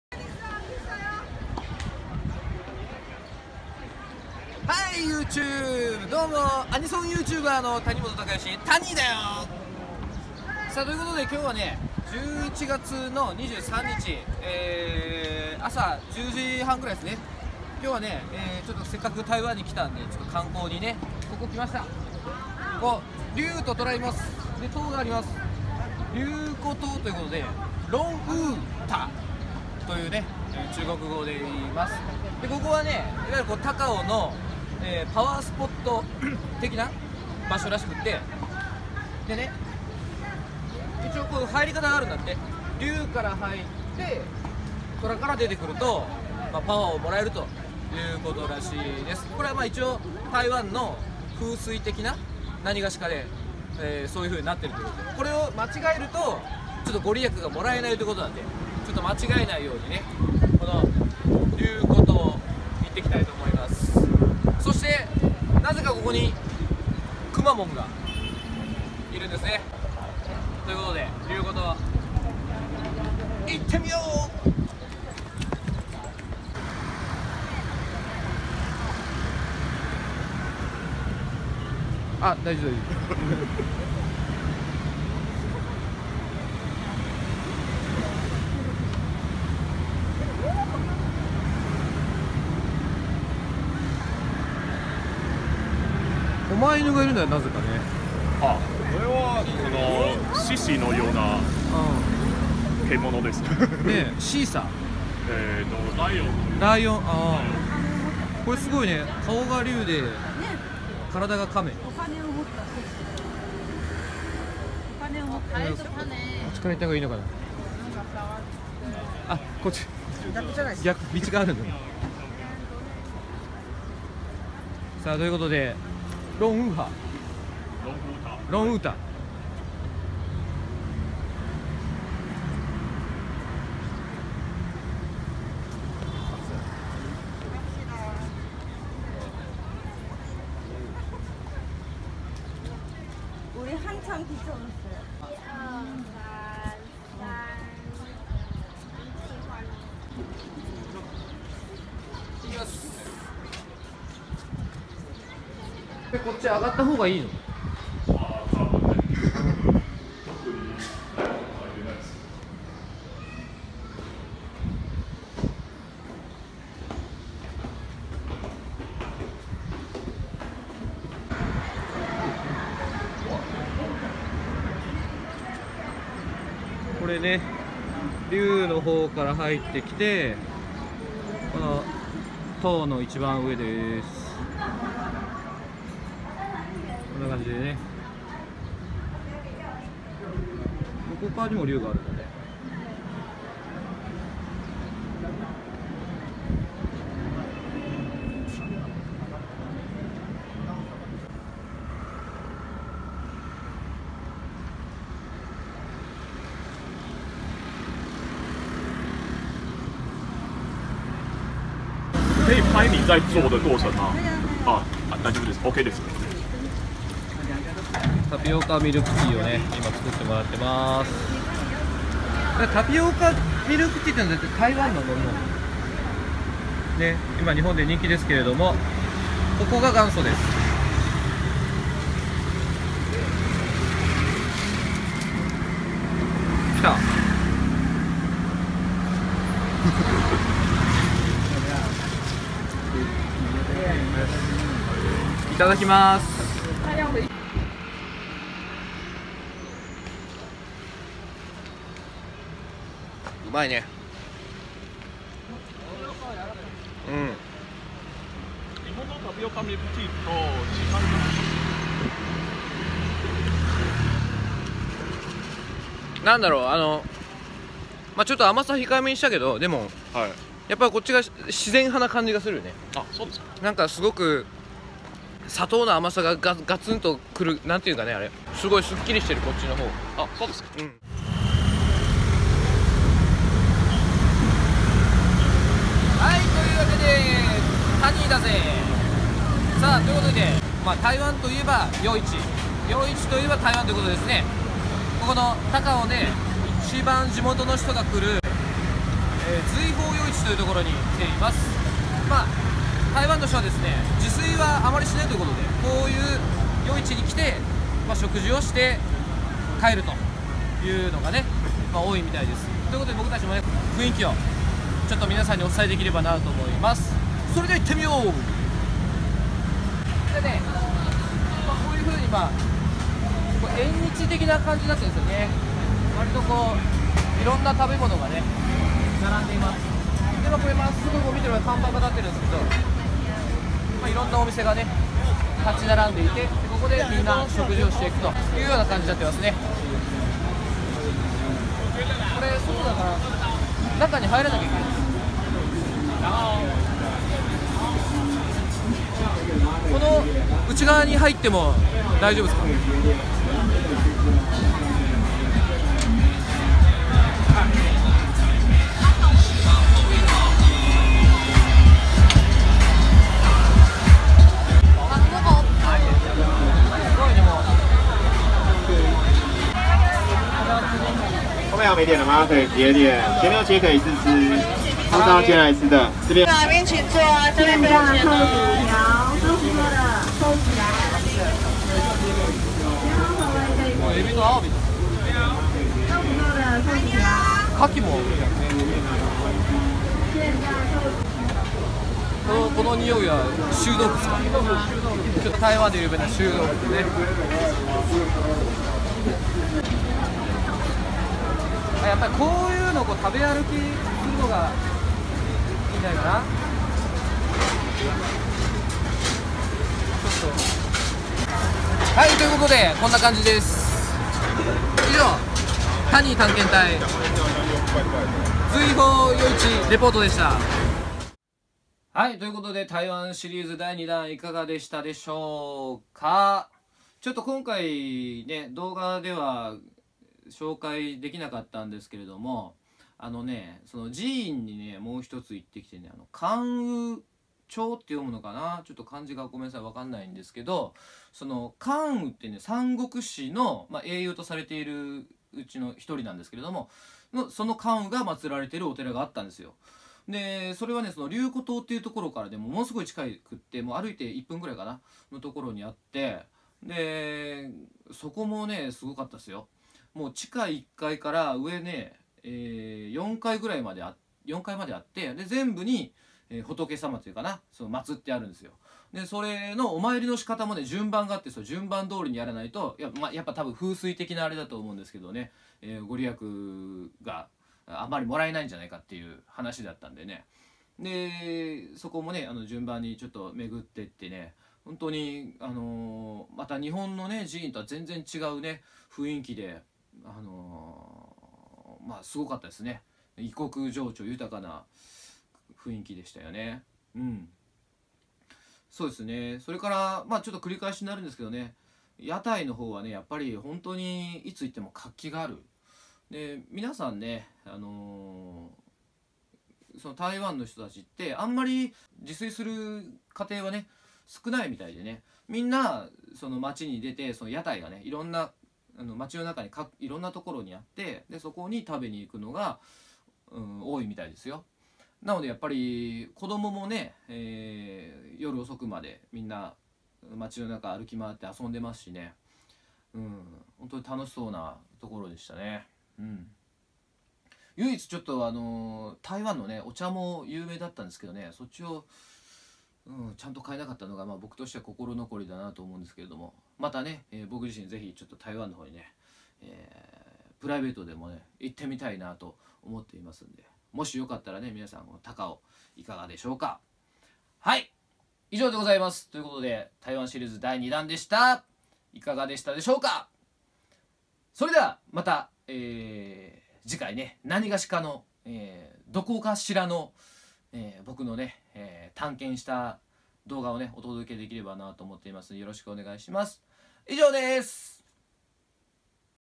タニー探検隊♪ 台湾 高雄編 その2
谷本貴義のYoutube channel

台湾高雄市で開催されたアニメコンベンション
「駁二動漫祭2019」ライブステージに出演しました。
台湾高雄の観光スポット紹介♪